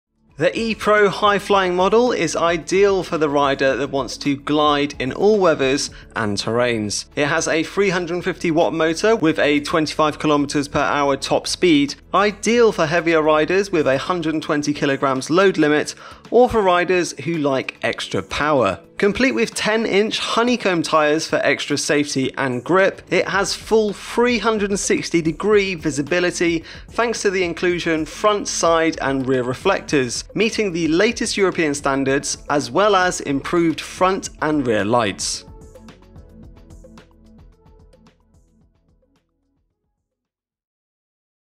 E PRO High Flying Electric Scooter - All Terrain Model (350w motor)
The E-Pro H10E is ideal for the rider that wants to glide in all weathers and ride on varying terrain.

It has a 350W motor with a 25km/h top speed. Ideal for heavier riders with a 120kg (19 Stone) load limit or riders who like extra power.
It has full 360-degree visibility thanks to the inclusion front, side and rear reflectors, meeting the latest European standards, as well as improved front and rear lights.

Technical Specs Hi-Flying

ECO-FLYING (H10E model)

Speed- Level 1: 15km/h, Level 2: 20km/h, 3rd Level: 25km/h

Maximum Speed - 25km/h

Ranger per charge - 20-35km

Climbing angle-  Slope 20°

Battery- 36V 10Ah lithium battery

Motor Power (Watts)- 350W brushless motor

Charge Time (hours) - 5-6H

Brakes- Front Brakes Rear Drum Brake + Front electric brake

Tyre Size- 10inch Honeycomb tyre

Lamp- Head lamp, real KBA tail Lamp

Folded Dimensions - 42 x 17 x 20 inches   (LxWxH) 110 x 42 x 52 cm

Unfolded Dimensions- 42 x 19 x 45 inches   (LxWxH) 110 x 42 x 115 cm

PSI - 50

Maximum Rider Weight- 120kg

Ground distance- 15cm

Certification- CE

Colour- Black

Weight - 13.5KG

Extra Features   License plate, Reflector, Bell   Handlebar grip, speedometer   Charging Adapter: EU/UK Standard Plug   350W High speed motor   Foldable   Highlight headlight   Quick folding and easy carrying   New intuitive  LED. Dashboard   Bright LED double light.